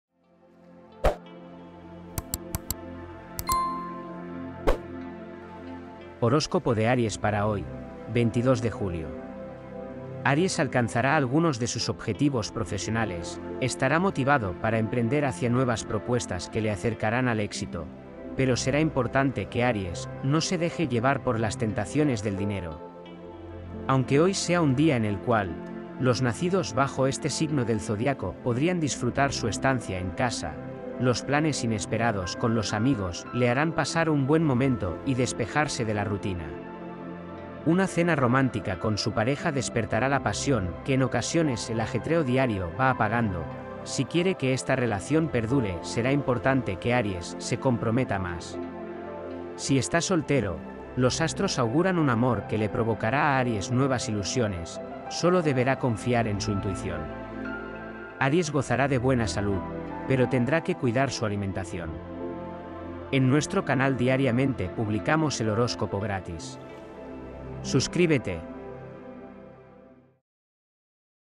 HORÓSCOPO DIARIO 🥰💖🎁 ARIES ♈️ JUEVES, 22 DE JULIO DE 2021
Horóscopo de hoy, jueves 22 de Julio de 2021. El horóscopo para hoy: descubre tu futuro gracias a mi horóscopo diario para ti.